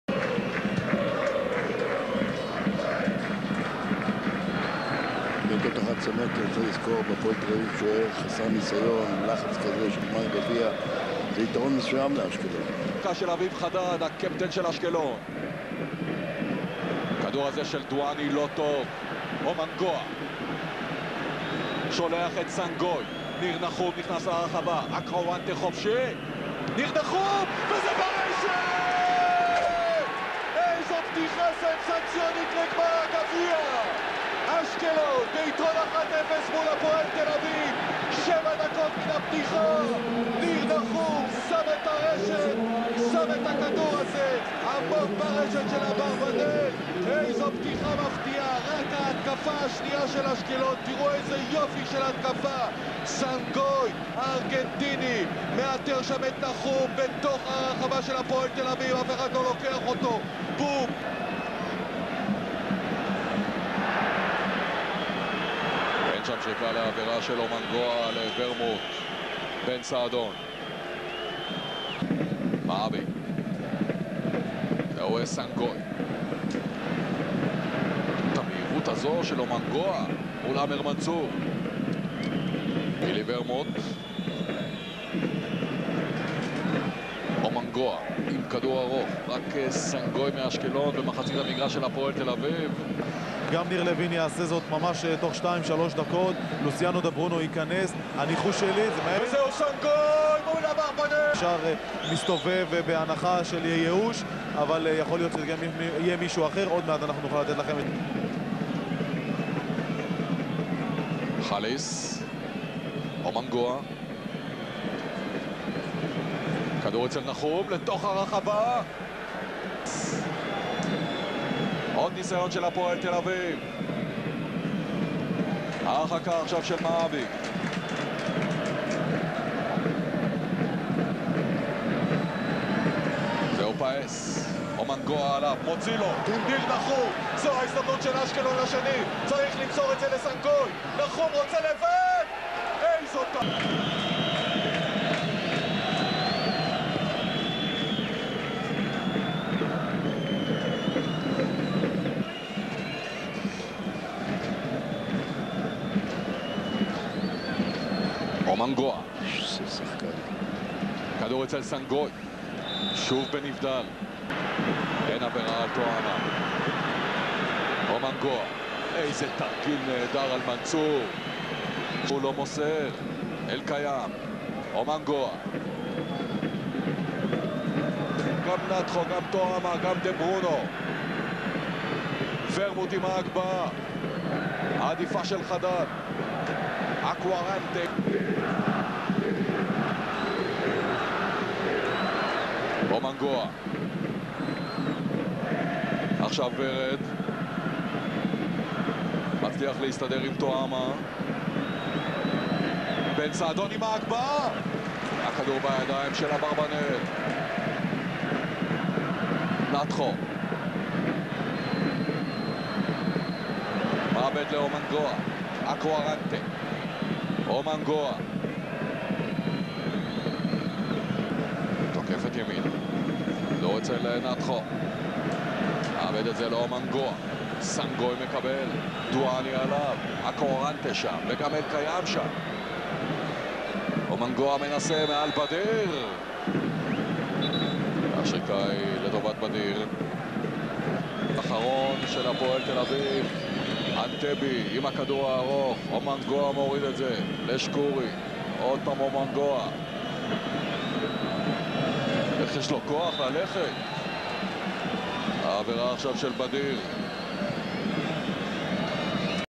AMUNGWA - Israel Cup Final 2007
Hapoel Tel Aviv  v  Ashkelon
Israeli Cup Final 16th May 2007
Highlights of Emmanuel Amungwa